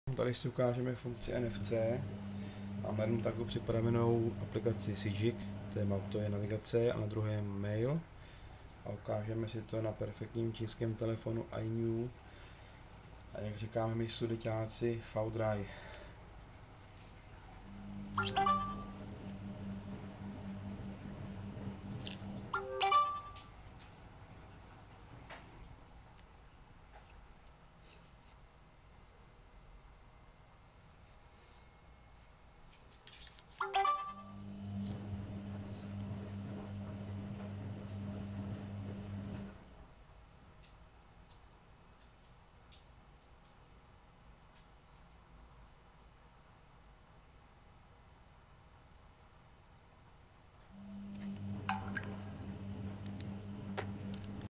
iNew V3 and NFC smart tags
Ukázka funkce NFC s lepíky smart tags na telefonu iNew V3